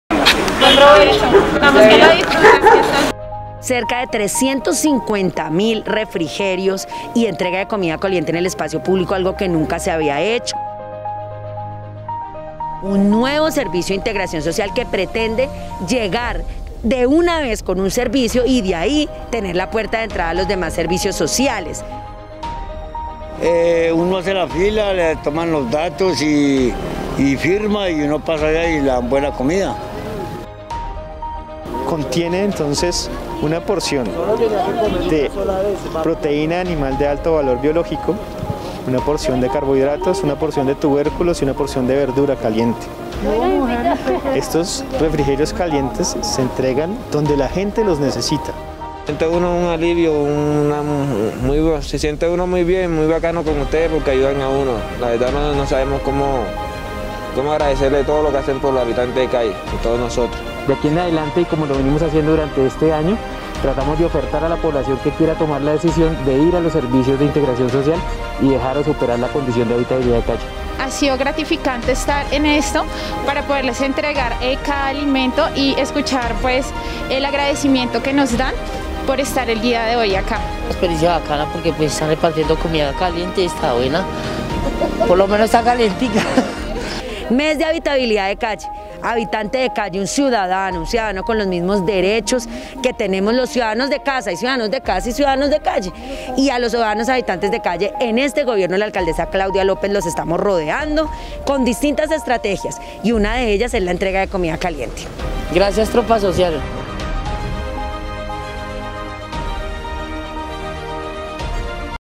La Tropa Social de Rescate Entrega comida caliente
La Tropa Social de Rescate se tomó las calles de la Plaza España, en la localidad de Los Mártires, para llevar refrigerios reforzados calientes a personas que viven situaciones de vulnerabilidad económica y social. Nuestra secretaria de Integración Social, Xinia Navarro.

Todos los días entregamos 8.000 raciones de refrigerios calientes reforzados a población vulnerable y en pobreza extrema, como medio para vincularlos a nuestros servicios de Integración Social